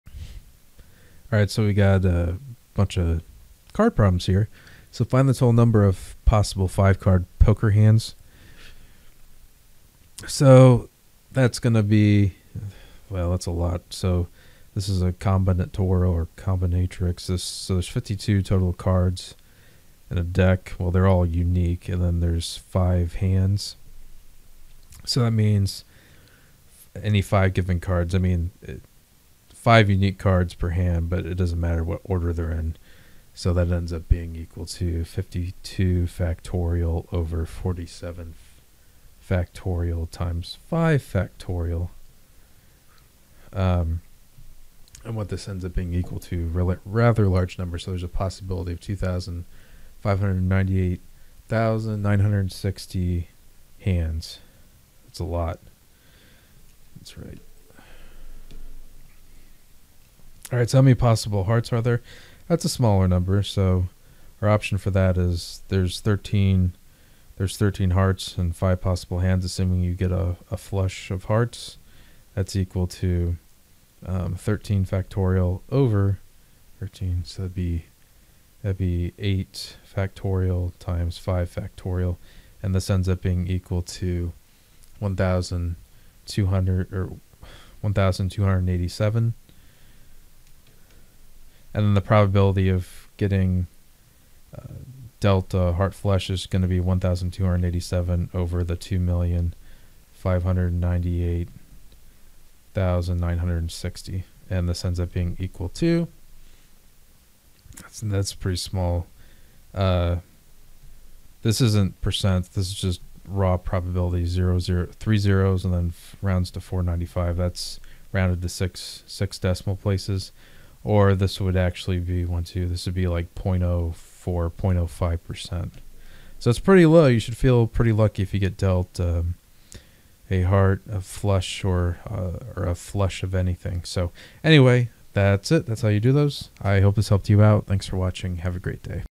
A hand consists of 5 cards from a well-shuffled deck of 52 cards. a. possible 5-card poker hands.
A hand consists of 5 cards from a well-shuffled deck of 52 cards.

a. Find the total number of possible 5-card poker hands.
b. A heart flush is a 5-card hand consisting of all heart cards. Find the number of possible heart flushes.
c. Find the probability of being dealt a heart flush.